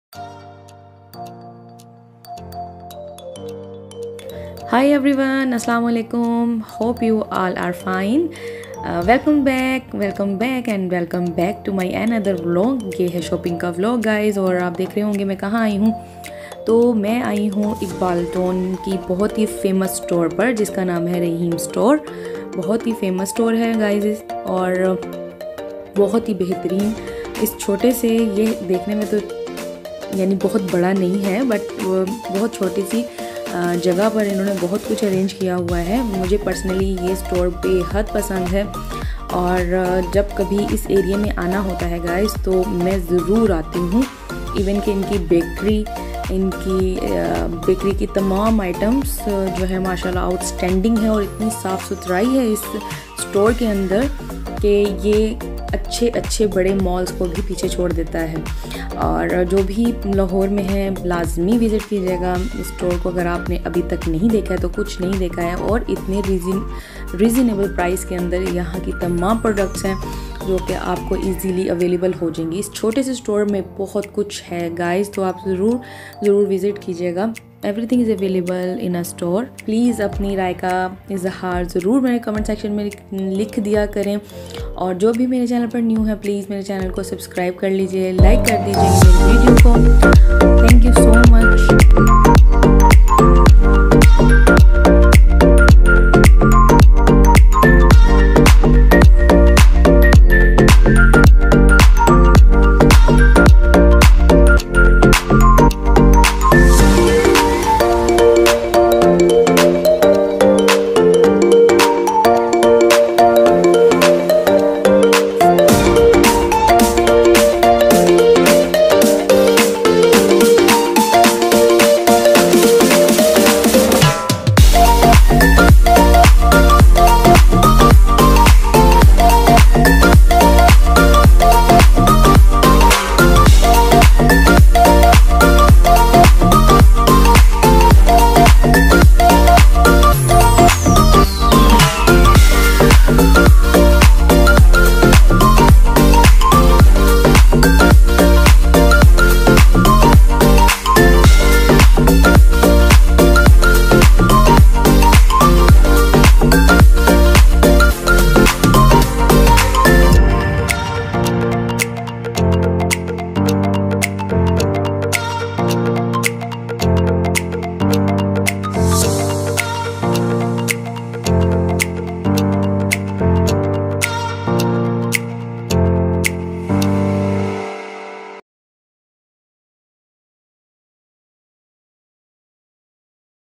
Shopping vlog 1 || Rahim Store || Iqbal Town Lahore || Lahore Pakistan || Full-HD
Assalam-O-Alykum guys!!
Hi friends today I'm sharing some shopping with you guys I hope u will definitely like the video and enjoyed a lot for more videos plz stay tuned on my channel weddingshopping bridaldresses bridalsandals centorousmall Islamabadshoppingmall#

Thanks